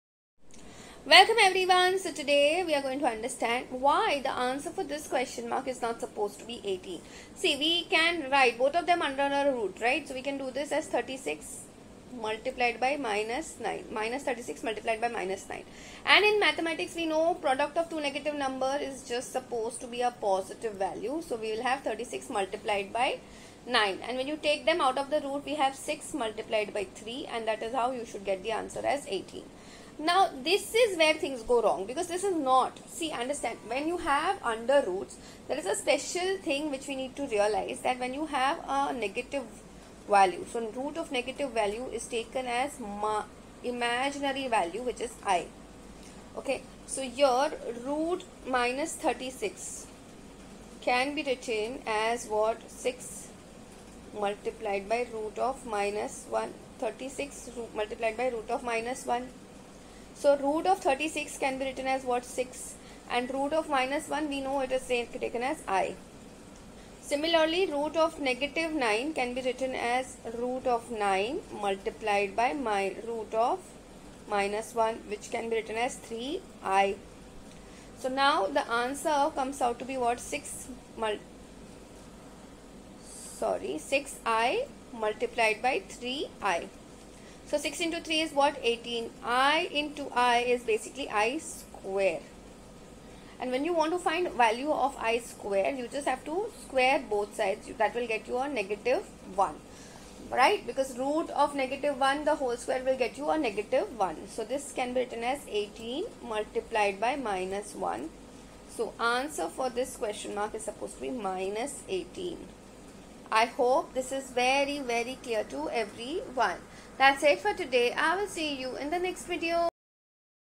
√-2 × √-8 Everyone thought the answer was 4 and got it wrong. Can you do it right?
which is right? square root of negative numbers multiplication.